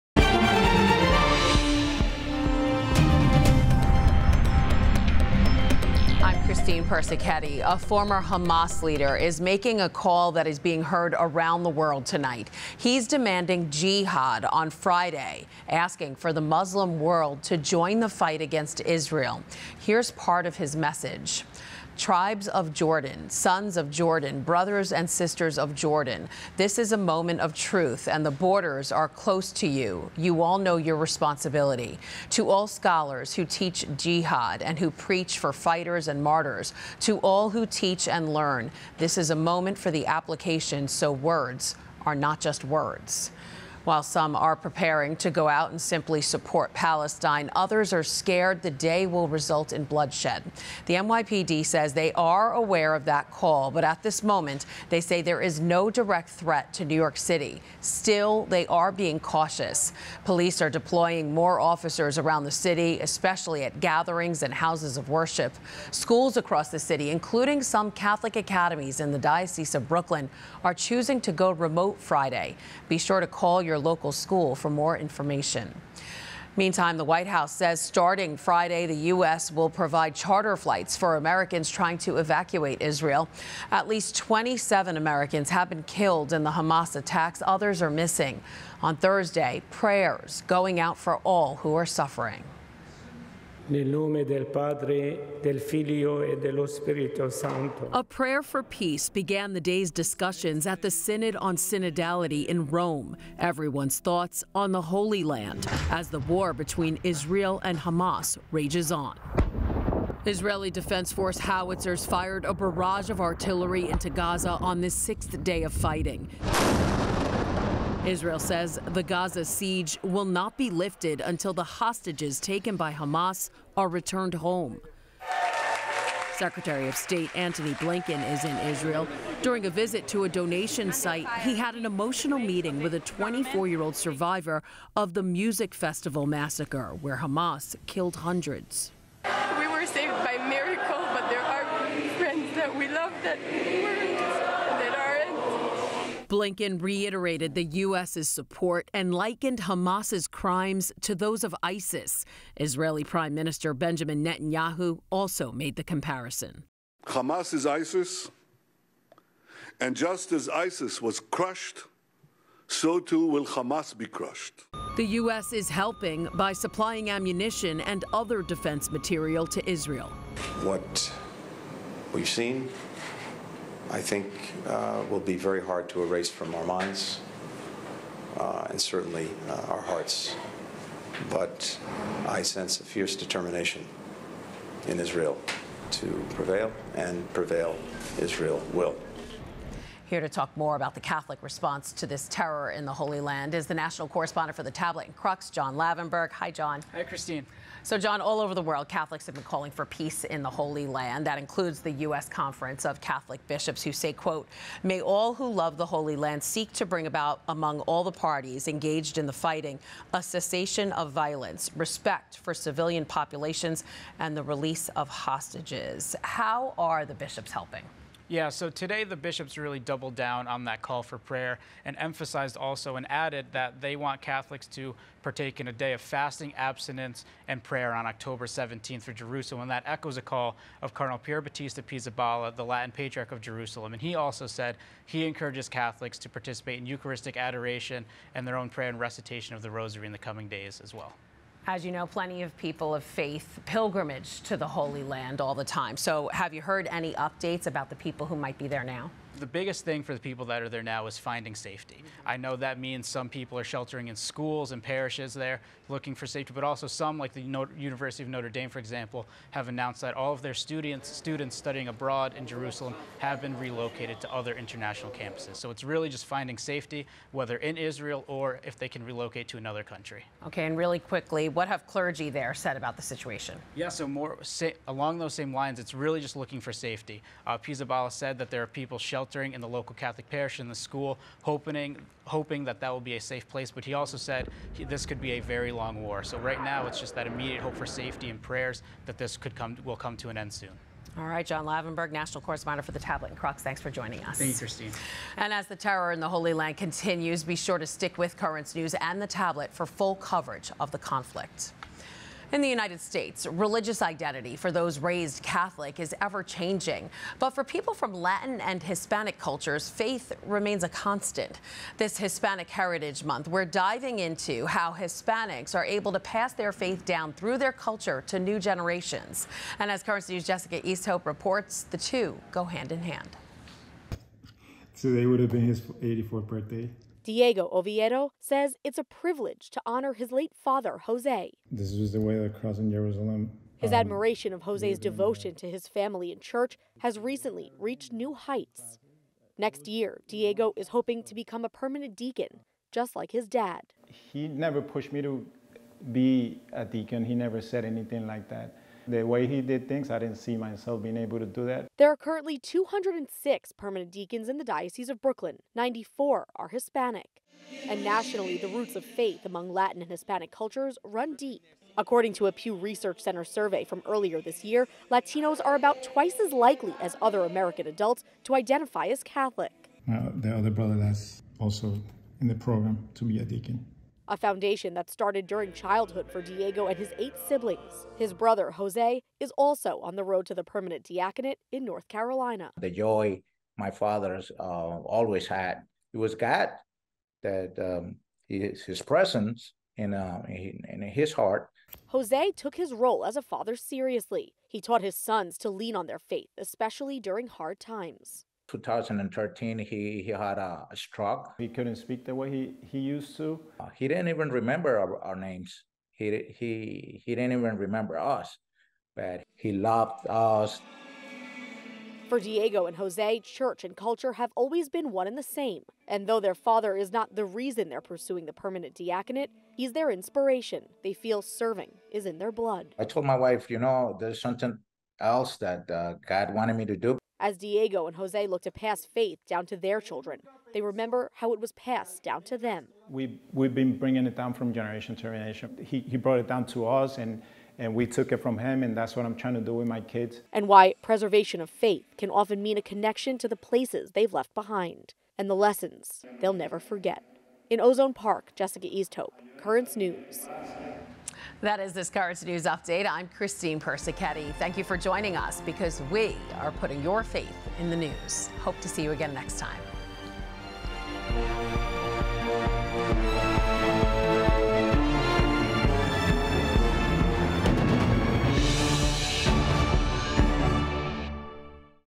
Catholic News Headlines for Thursday 10/12/2023
The death toll continues to mount in Israel and Gaza as the war rages on.

Tonight we're diving into how Hispanics are able to pass their faith down, through their culture, to new generations.